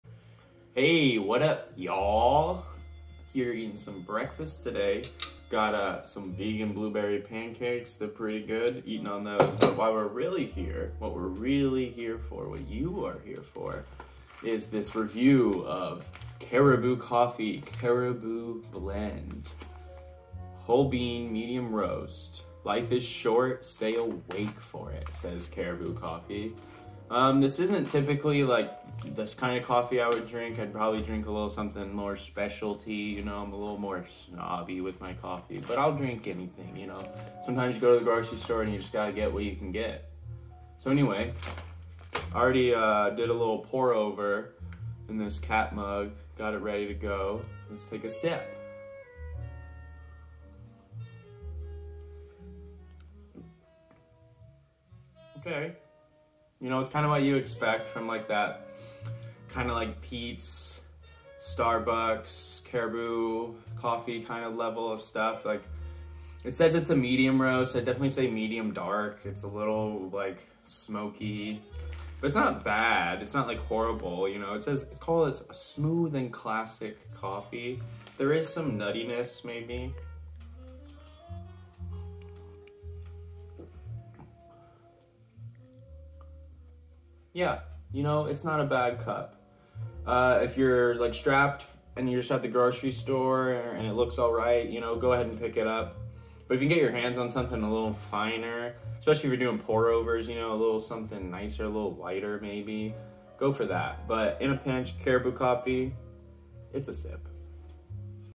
Caribou Coffee "Caribou Blend" Review
Lil breakfast time review of The Caribou Blend from Caribou Coffee. It is a medium roast. Brewed this bad boy up on a Hario v60. Let me know if you want to know more about how I made it. I'll probably do a pourover something video soon.